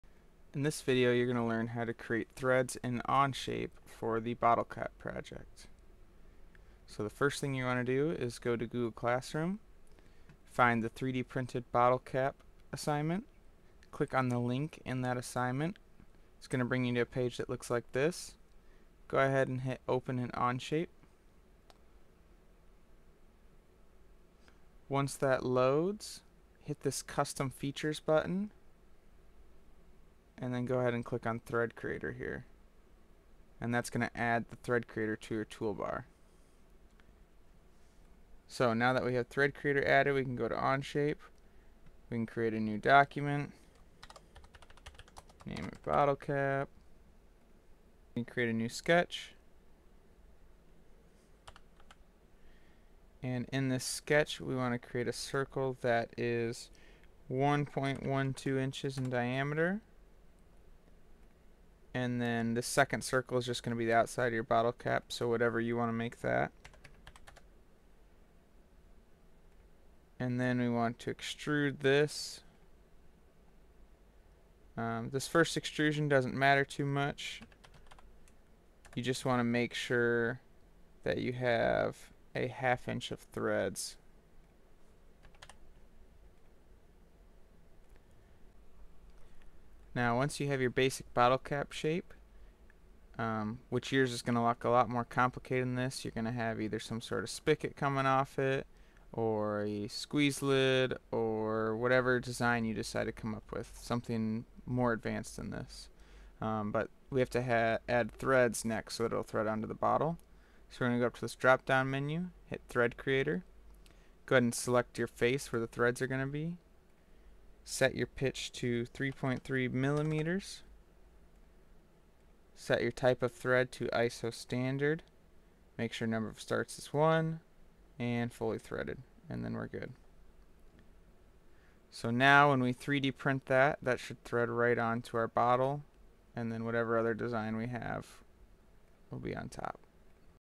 Creating Threads on OnShape/Bottle Cap Project
In this video you will learn how to create threads on OnShape for the Bottle Cap project.